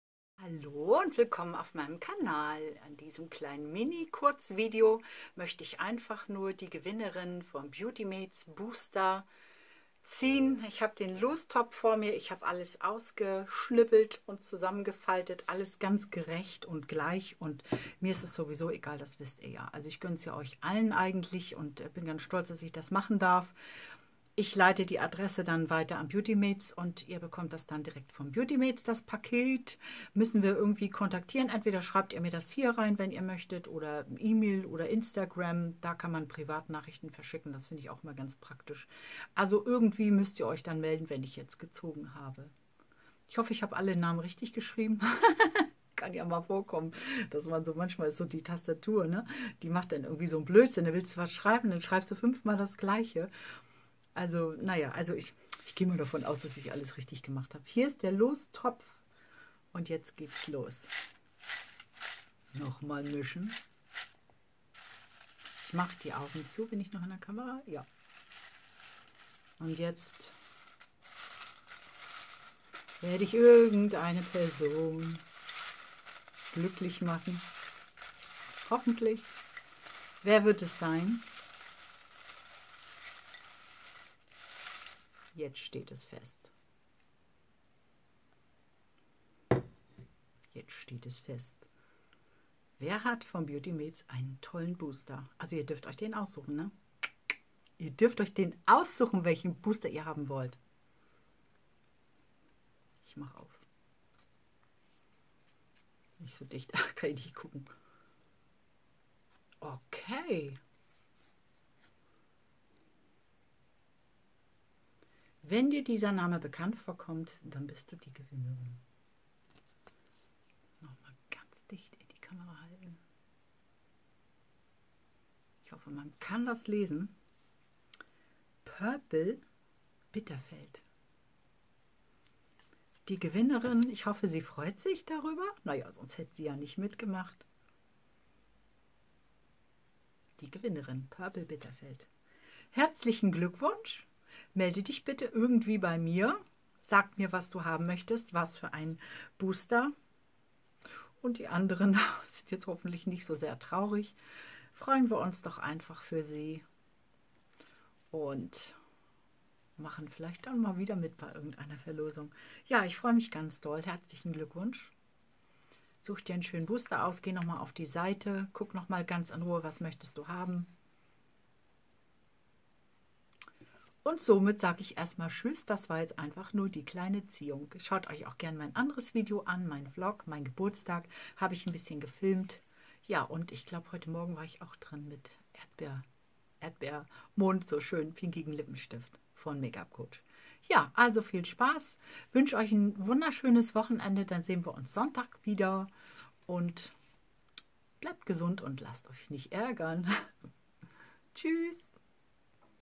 Gewinner Ziehung Beautymates Booster
Hallo,
in diesem Video ziehe ich den oder die Gewinner/in. des Beautymates Booster.

Per Email oder Instagram, bitte melden wegen der Adresse. Beautymates sendet es direkt zu Euch.

Viel Spass beim zuschauen wünscht Daniela vom Kanal SharoneS